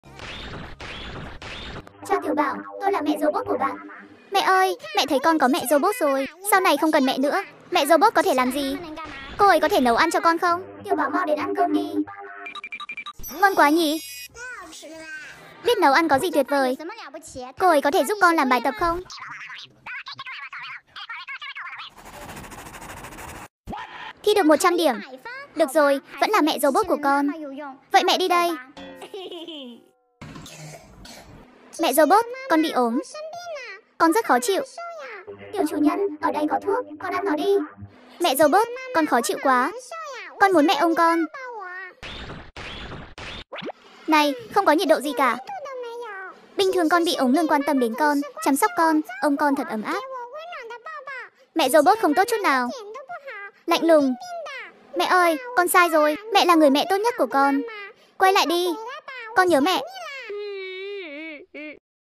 Tiểu Bảo có mẹ Robot và đuổi mẹ thật của Mình ra khỏi nhà
Đây là phim hoạt hình giáo trẻ em nhận biết tốt xấu, giải trí học hỏi,hài hước.